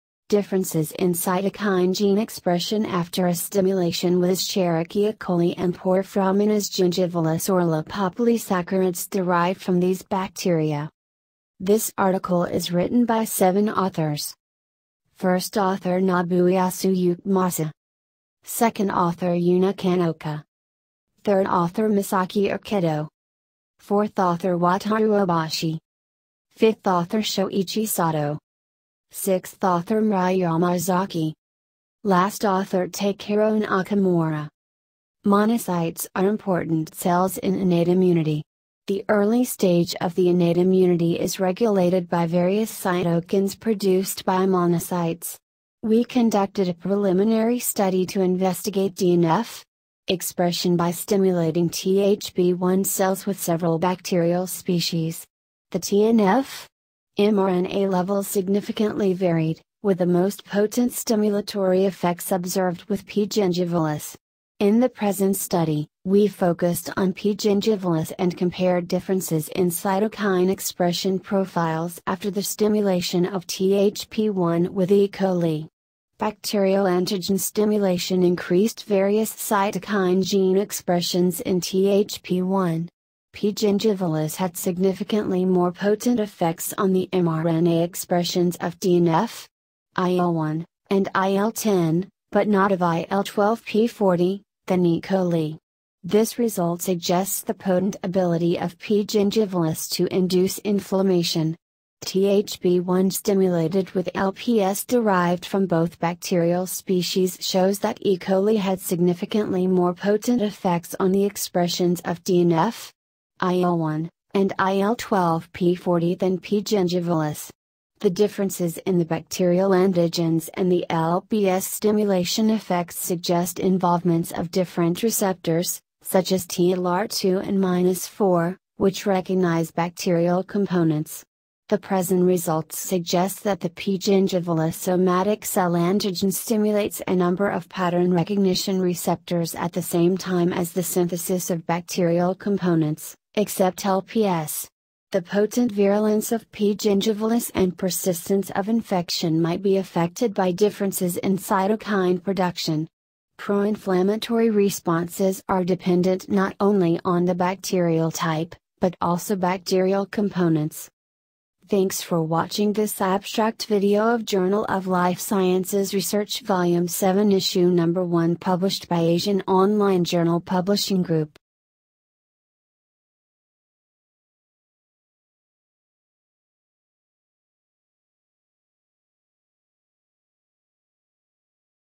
Differences in Cytokine Gene Expression after a Stimulation with Escherichia Coli and Porphyromonas
Differences in Cytokine Gene Expression after a Stimulation with Escherichia Coli and Porphyromonas Gingivalis or Lipopolysaccharides Derived from these Bacteria